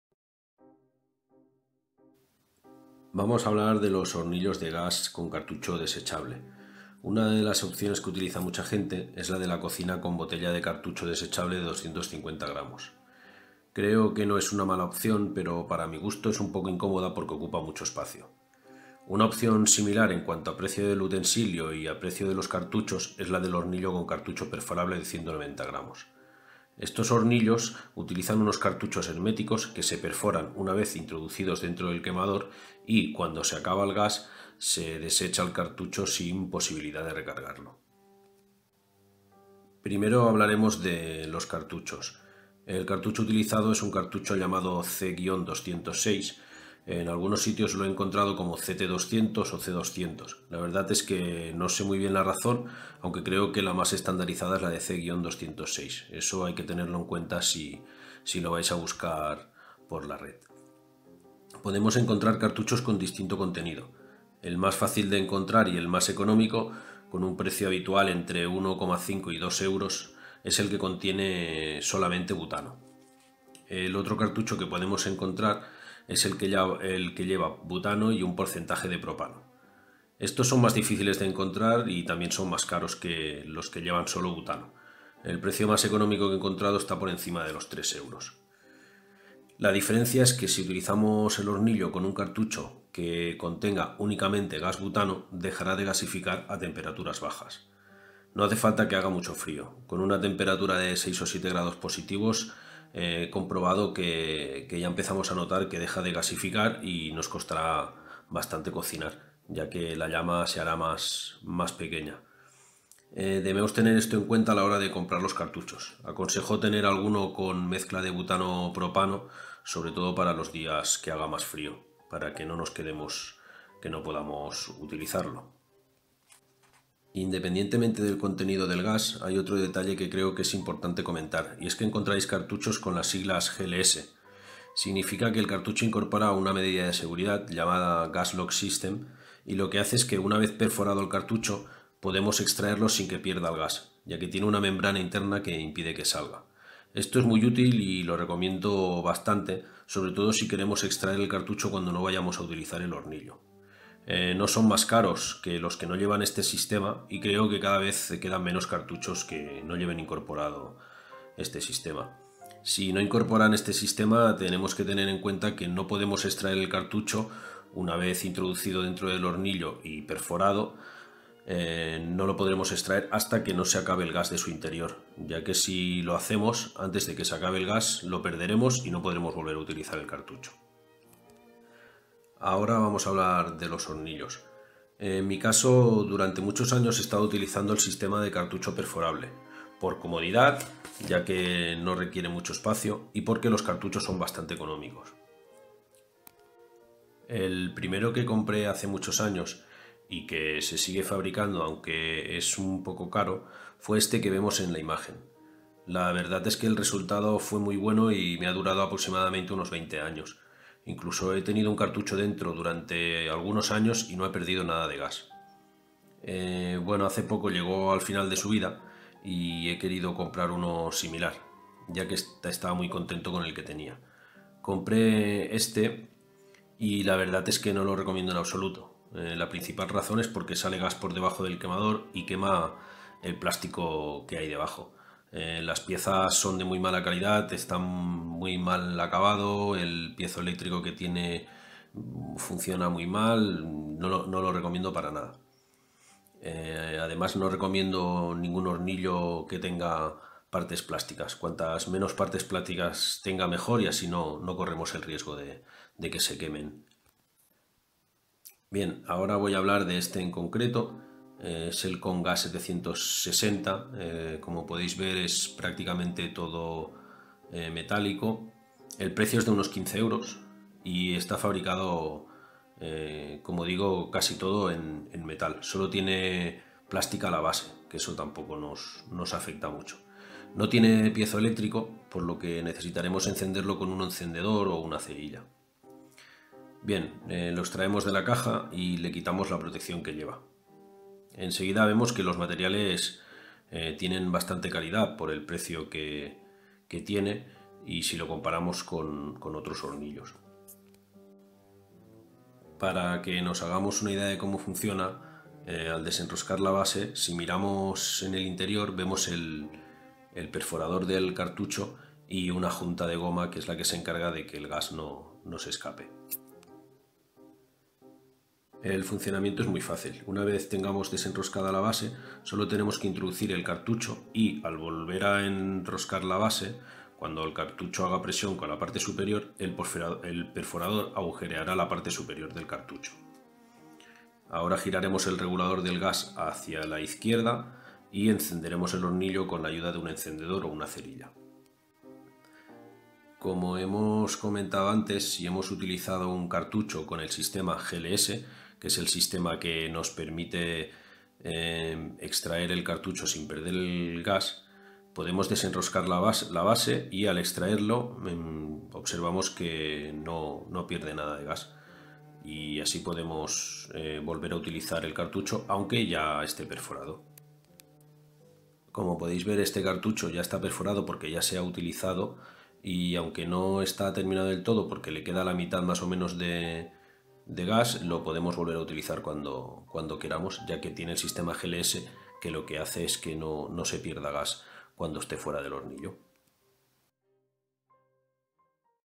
BricoPimPam - 10 - Hornillo de camping con cartucho desechable de 190 g. (15 Euros)
Explicación funcionamiento cartuchos C-206 C 206 C190 C200 CT-200 con 190 gramos de contenido. Butano y mezcla butano/propano.
Comparativa, análisis, unboxing hornillo COM GAS 760.